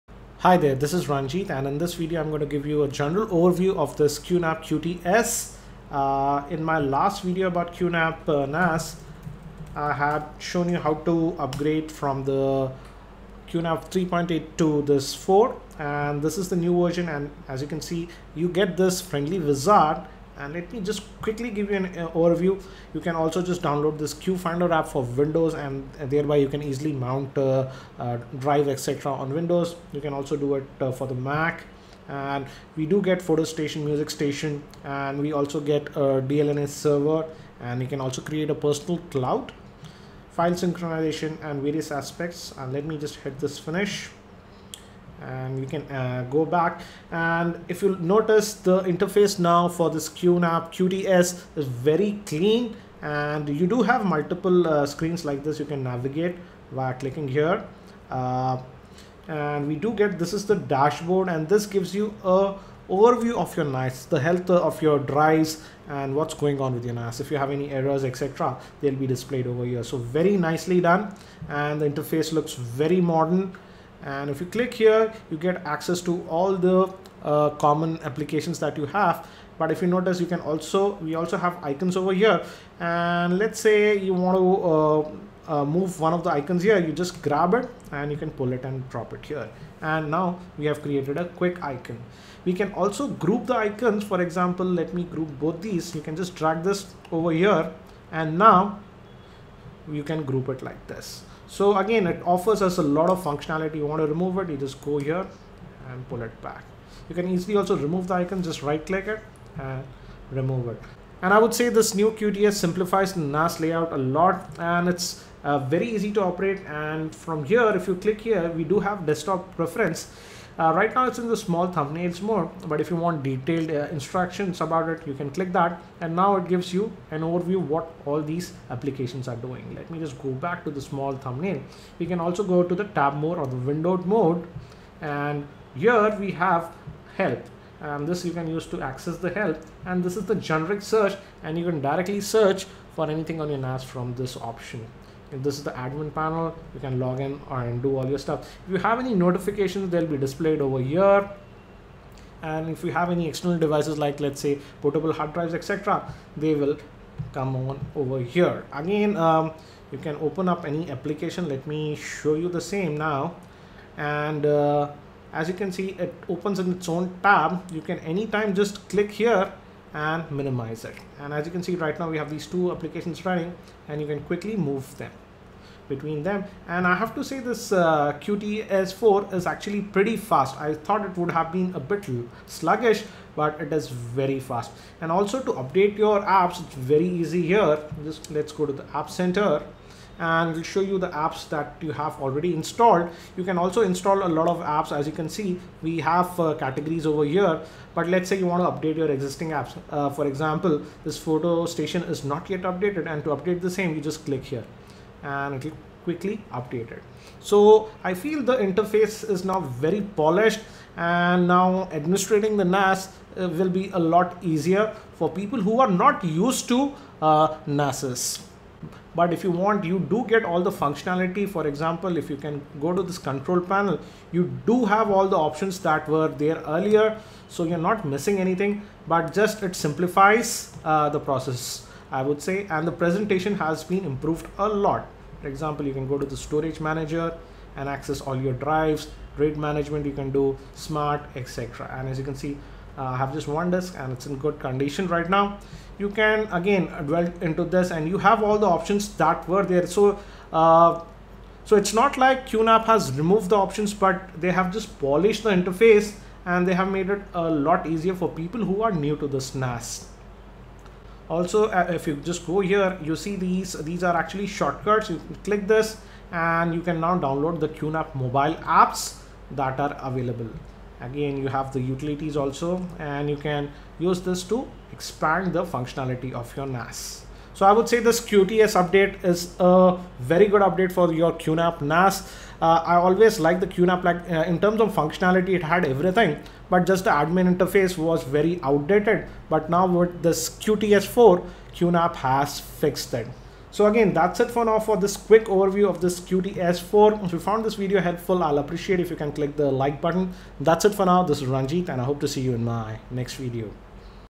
QNAP NAS New QTS 4.0 Overview
Overview of the New firmware QTS 4.0 for QNAP NAS, the QTS 4.0 brings a refreshed and easy to use user interface to the QNAP NAS as it looks very modern and I give you an hands on overview of this new QTS 4.0 update for the QNAP Nas.